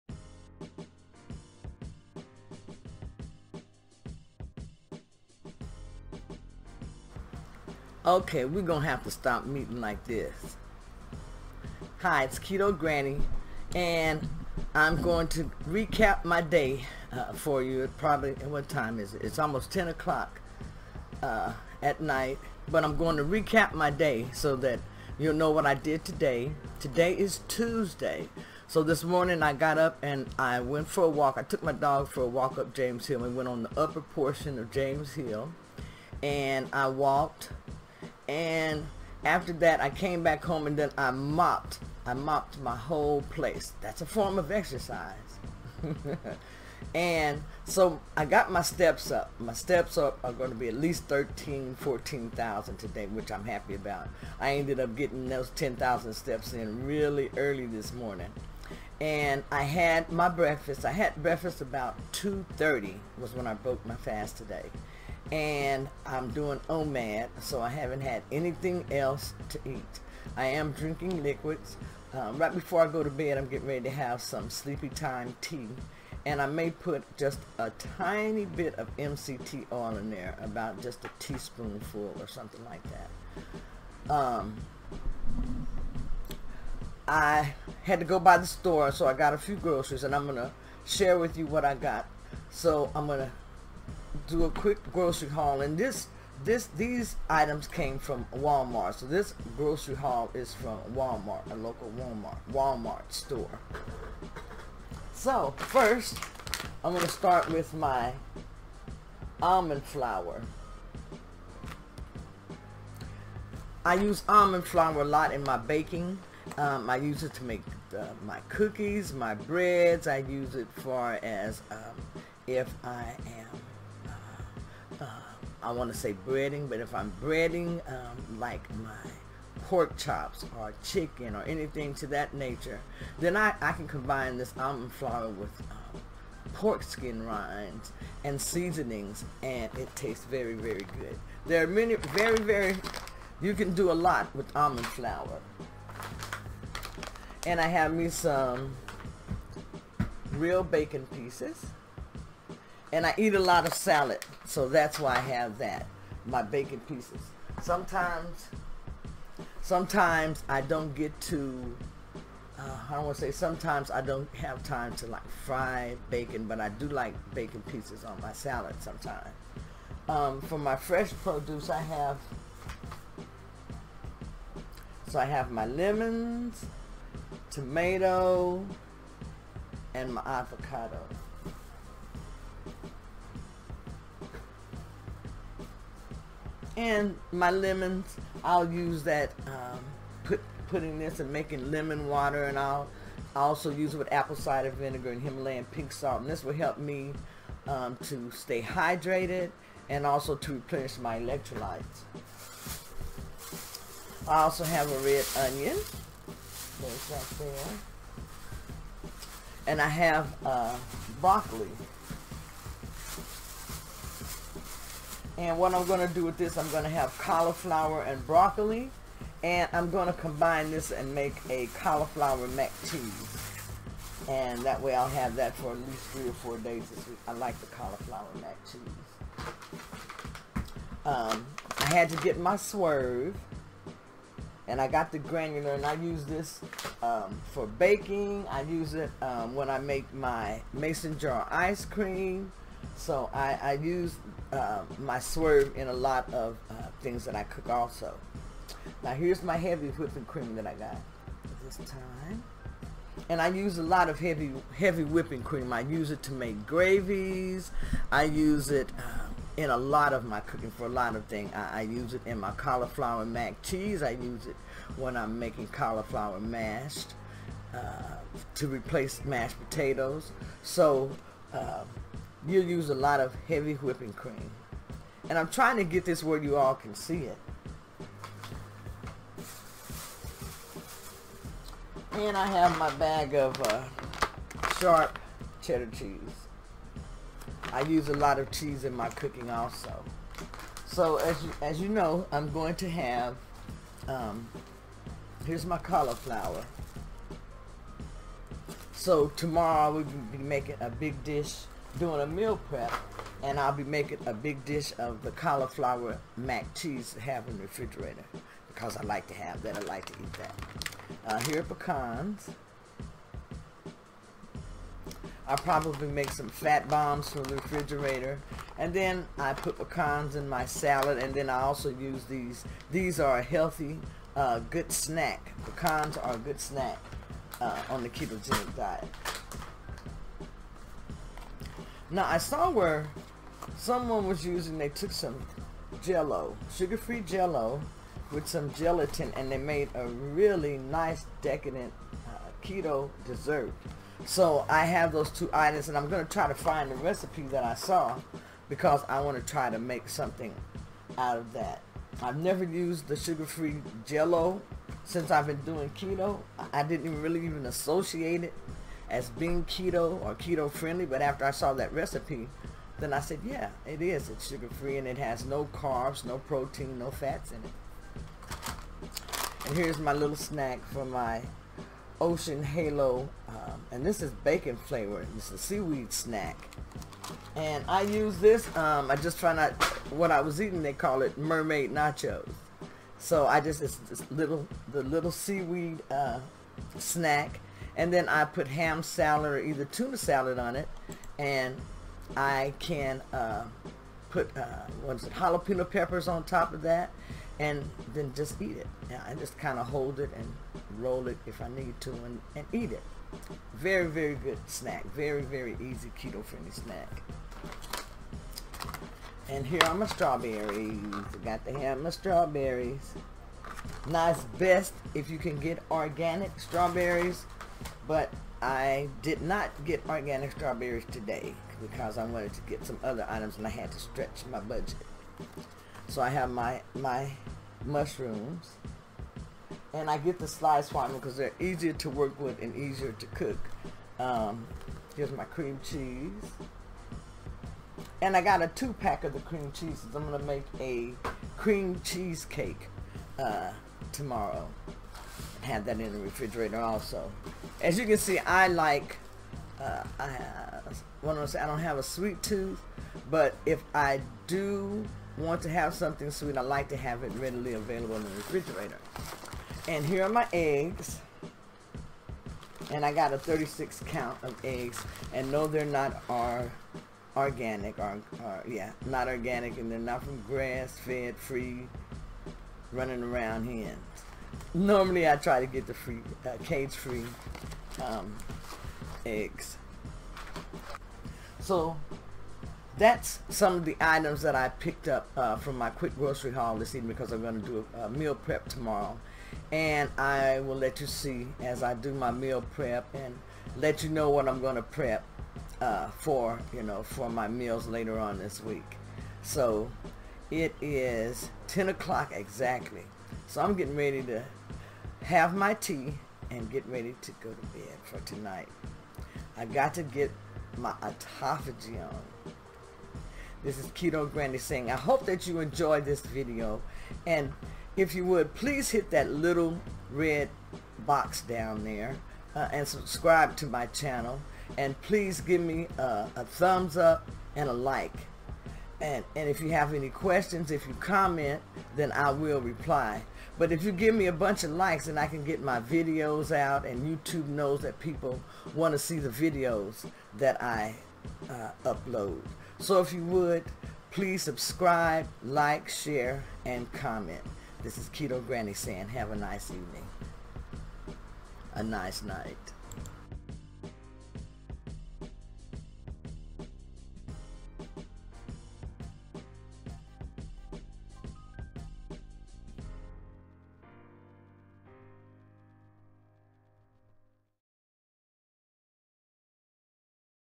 My Walmart Keto Grocery Haul
You can buy lots of keto/low carb foods from Walmart.  It's a budget friendly place to shop.  Here is what I picked up today for a quick grocery haul and meal prep.